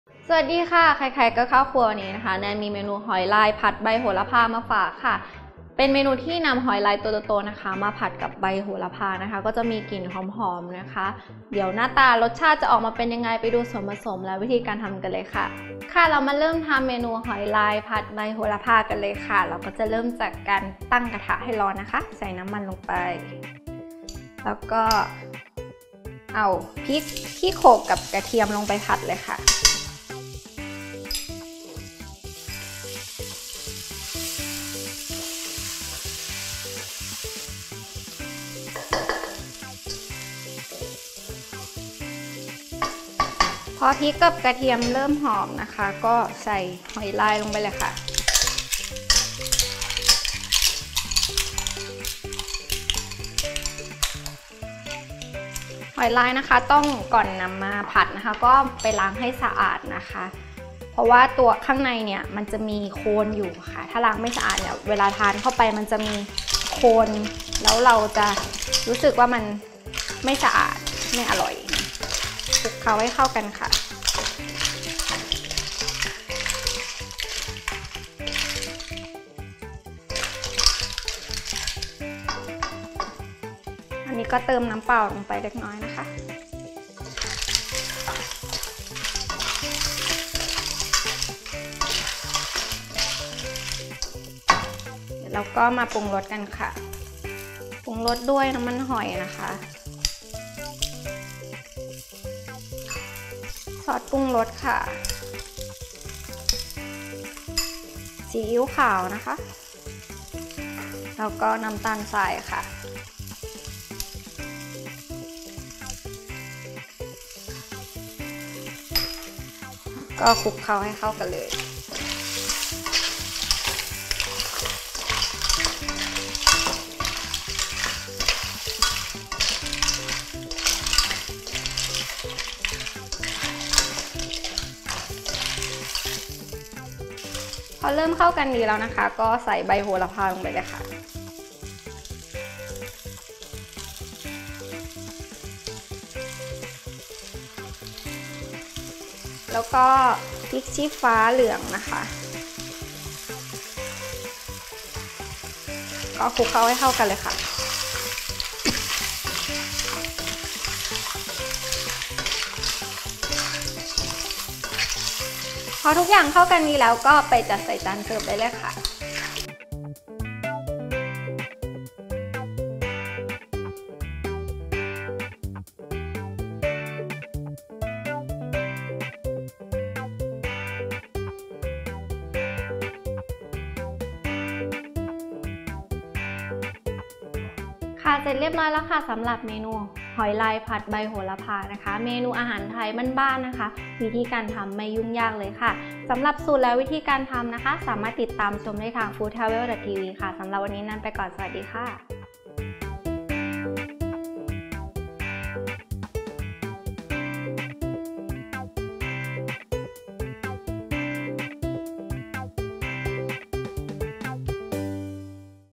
หอยลายผัดใบโหระพา Stir Fried Clams with Basil | 1 Minute Cooking
หอยลายผัดใบโหระพา Stir Fried Clams with Basil เมนูนี้แนนเชื่อว่าเป็นเมนูโปรดของหลายๆ คนแน่นอน ไม่ว่าจะทานกับข้าวสวยหรือข้าวต้มก็อร่อย ที่สำคัญทำง่ายด้วยค่ะ

วัตถุดิบหอยลายผัดใบโหระพา Stir Fried Clams with Basil

1. หอยลาย 200 กรัม
2. กระเทียม 4 กลีบ
3. พริกสดสีแดง 5 เม็ด
4. น้ำมันหอย 1 ช้อนโต๊ะ
5. ซีอิ๊วขาว 1 ช้อนชา
6. ซอสปรุงรส 1/2 ช้อนชา
7. น้ำตาลทราย 1 ช้อนชา
8. น้ำเปล่า 2 ช้อนโต๊ะ
9. พริกชี้ฟ้าสีเหลืองหั่นแฉลบ 1/2 เม็ด
10. น้ำมันพืช 1 ช้อนโต๊ะ
11. ใบโหระพา 1/2 ถ้วยตวง

กระทะ

วิธีทำหอยลายผัดใบโหระพา Stir Fried Clams with Basil
1.  โขลกกระเทียม พริกสด พอแหลก ตักขึ้นพักไว้
2.  ตั้งกระทะใส่น้ำมันพอร้อน ใส่เครื่องที่โขลกไว้ลงผัดพอหอม ใส่หอยลายผัดพอสุก
3.  ปรุงรสด้วยน้ำมันหอย ซีอิ๊วขาว ซอสปรุงรส น้ำตาลทราย เติมน้ำเปล่า ลงผัดพอเข้ากัน ใส่ใบโหระพา พริกชี้ฟ้า ลงผัดพอเข้ากัน ตักใส่ภาชนะ จัดเสิร์ฟ

Ingredients

1-2 serve

1. Clams 200 g.
2. Garlic 4 cloves
3. Red chili 5
4. Oyster sauce 1 tbsp.
5. Soy sauce 1 tsp.
6. Seasoning sauce ½ tsp.
7. Sugar 1 tsp.
8. Water 2 tbsp.
9. Yellow chili 1/2
10. Oil 1 tbsp.
11. Basil 1/2 cup.